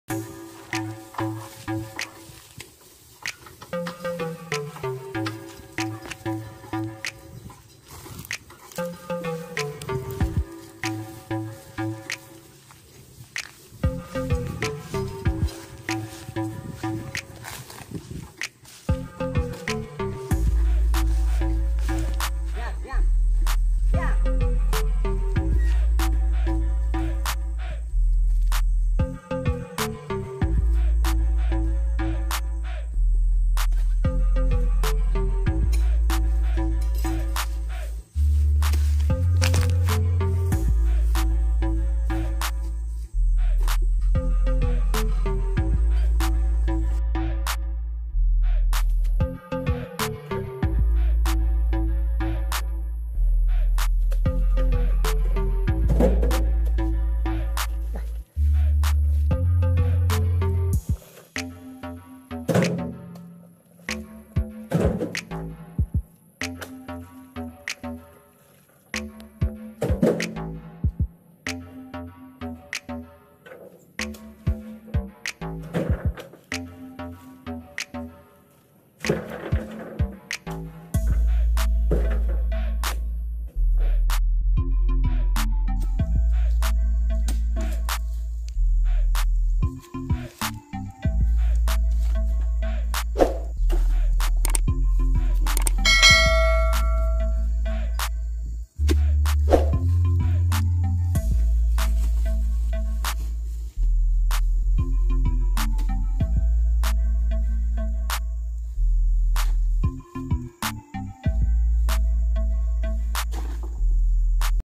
Muat sawit ditemani kerbau kesayangan
melihat aktivitas muat sawit hasil panen ,pemuat ini bisa di bilang muat sawit skil dewa karena buah yag di angkat termasuk buah sawit jumbo,semua dilakukan sendiri karena terbiasa sehingga aktivitas muat buah kelapa sawit sangat ringan bila diperhatikan